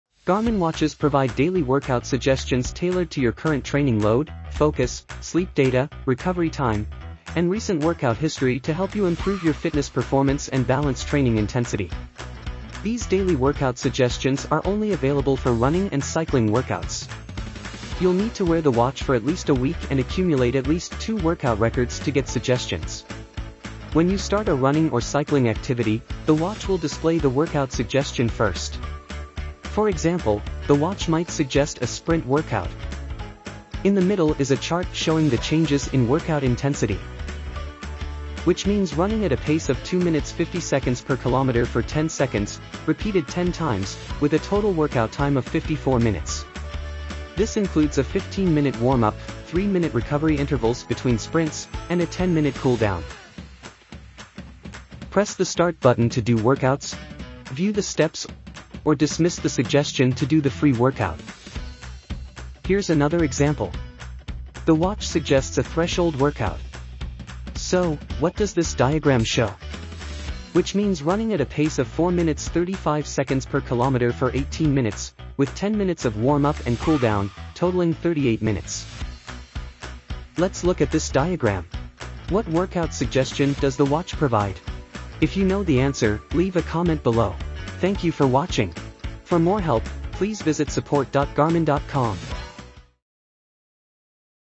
【仕様】今日のおすすめワークアウトの概要
今日のおすすめワークアウトは、フィットネスパフォーマンスを向上させ、体調に基づいた特定の目標を達成するために、スマートウォッチが自動的に推奨するワークアウトです。これらのおすすめワークアウトにより、現在のフィットネスレベルを維持または向上させ、全体的な成果を高めることができます。このビデオでは、スマートウォッチの今日のおすすめワークアウトグラフの数字の意味を解析します。

▶目次リンク
0:00　ビデオ紹介
0:24　今日のおすすめワークアウトの概要
0:30　例：10x0:10@2:50/km 54:00
1:06　例：18:00@4:35/km 38:00
1:22　宿題
1:28　エンディング

▶関連文章とビデオ
今日のおすすめワークアウト機能

詳細はGarminサポートセンターホームページをご参照ください